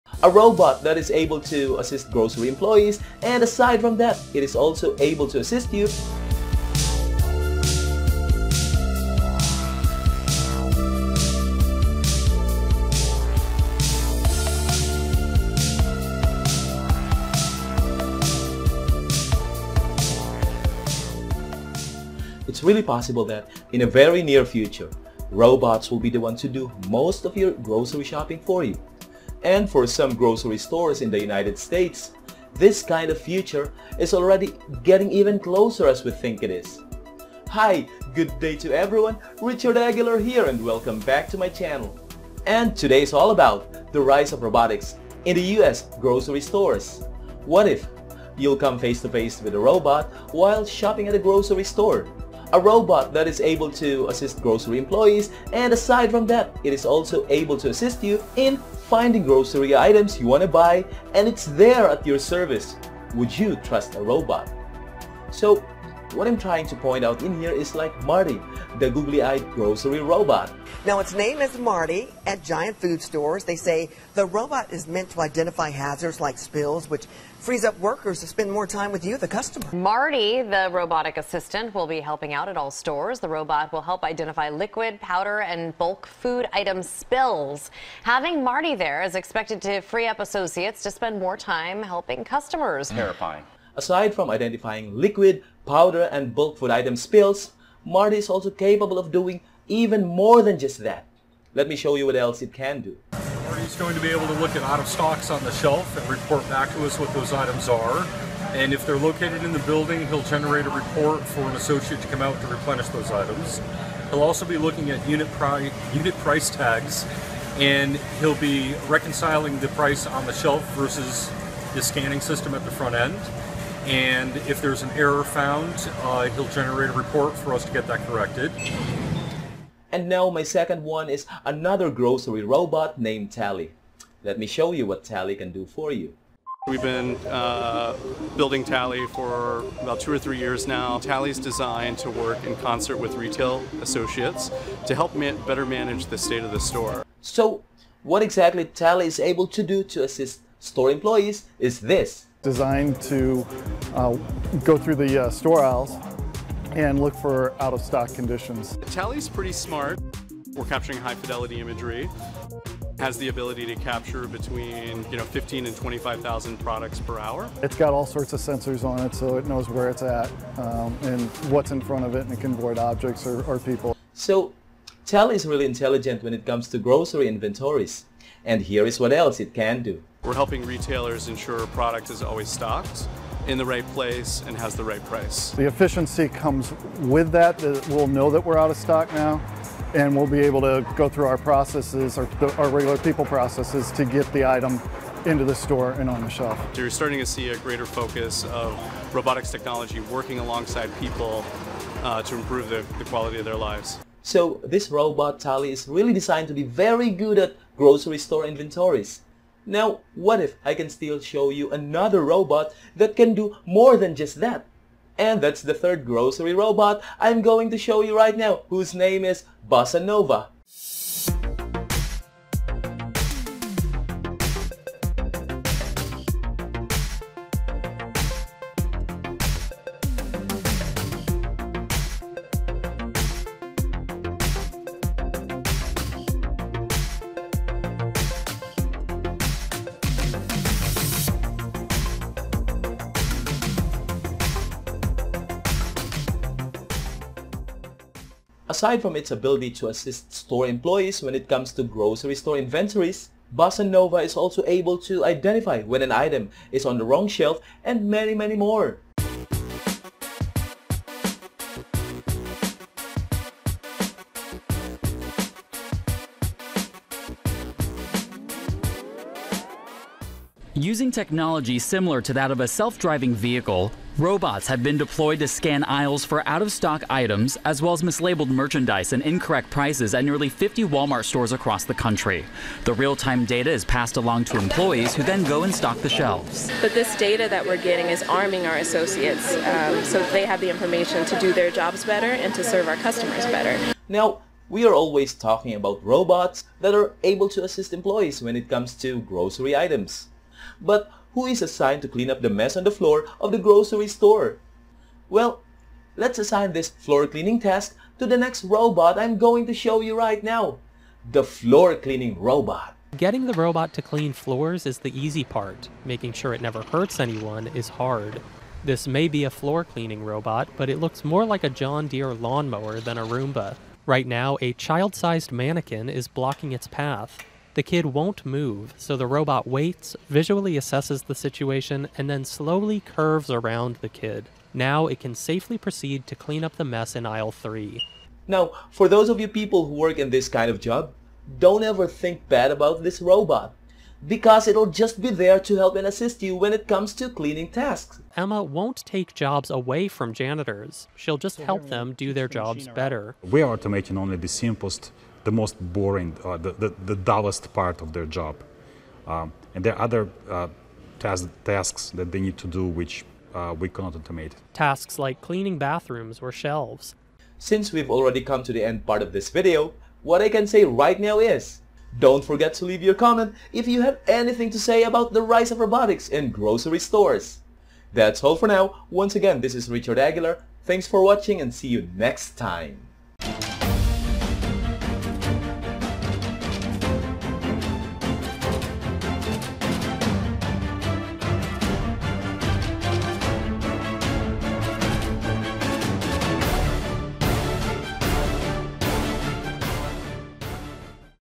The Rise of Robotics in The US Grocery Stores Is Unstoppable
What if you’ll come face to face with a robot while shopping at a grocery store? A robot that is able to assist grocery employees. And aside from that, it is also able to you in finding the grocery items you wanna buy and it’s there at your service. Would you trust a robot?

This Video is all about The Rise of Robotics in The US Grocery Stores

1. Marty – the Googly-Eyed Grocery Robot
2. Tally The Grocery Robot
3. Bossa Nova Robot
4. The Floor Cleaning Robot